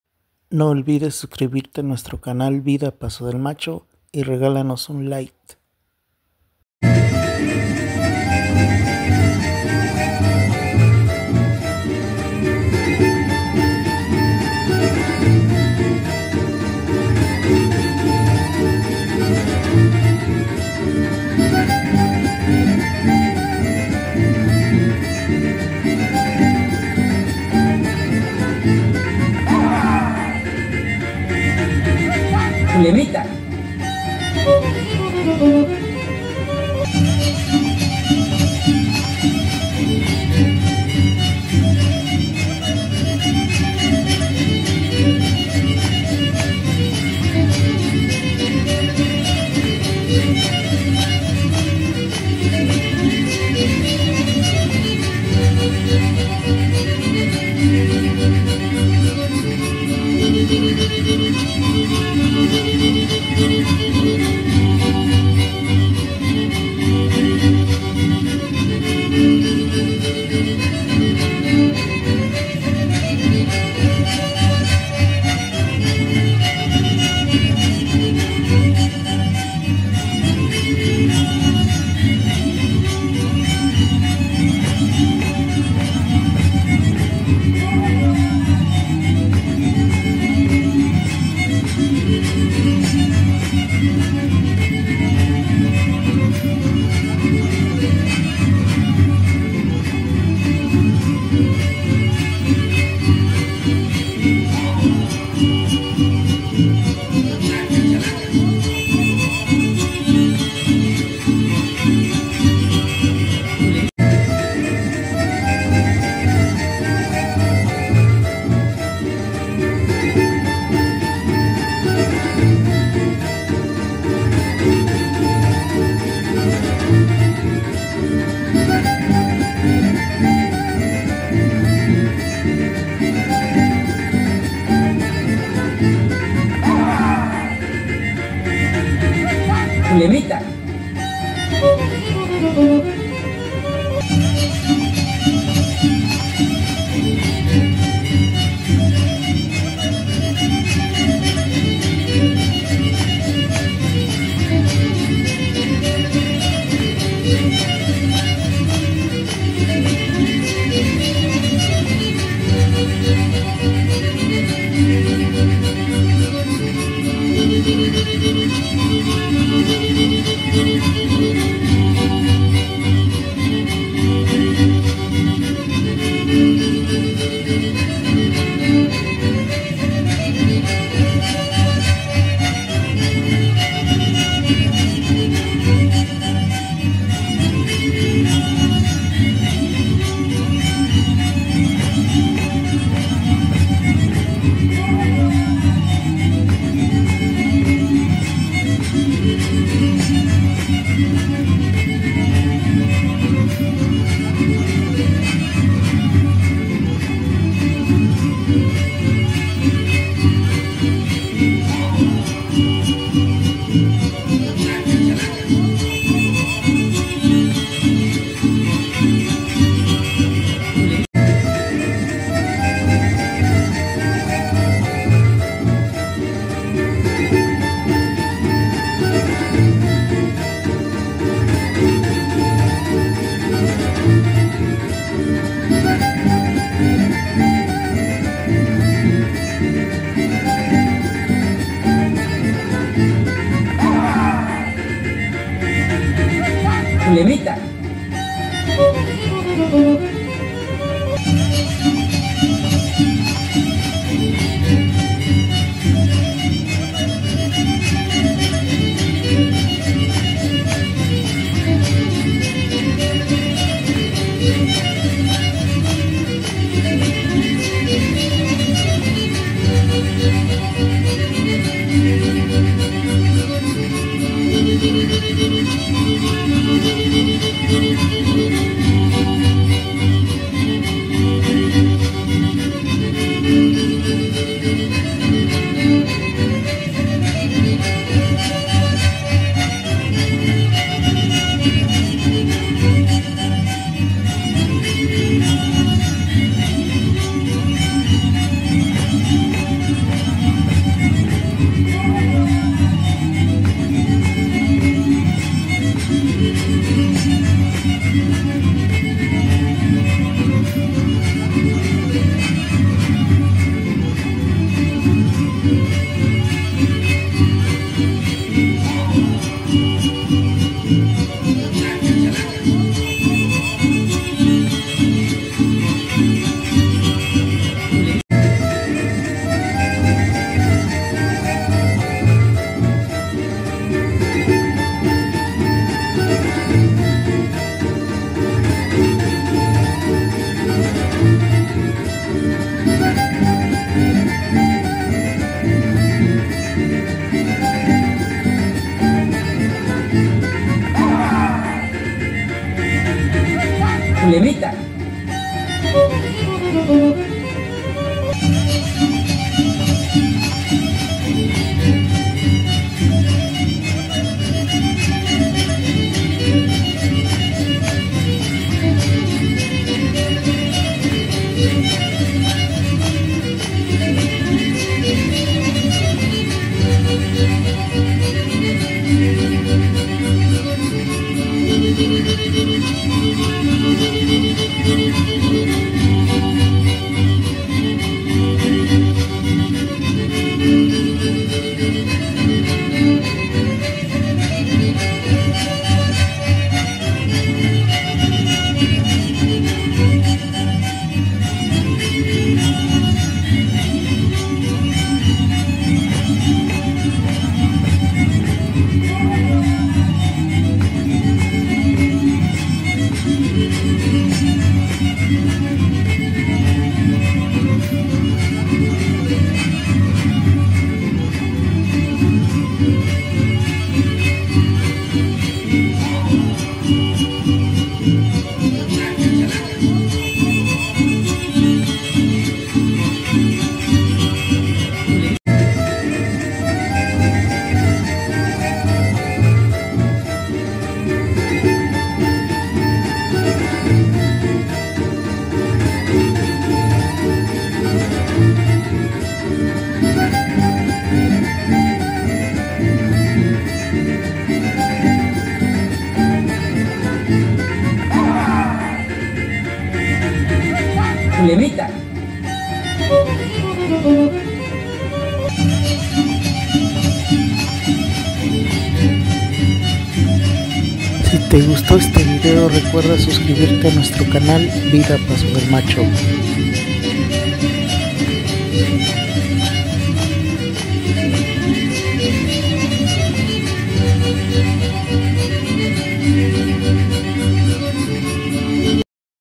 Primer Desfile de Cuadrillas Xantolo 2024
Se realizó el recorrido con la participación de jóvenes y autoridades de pasodelmacho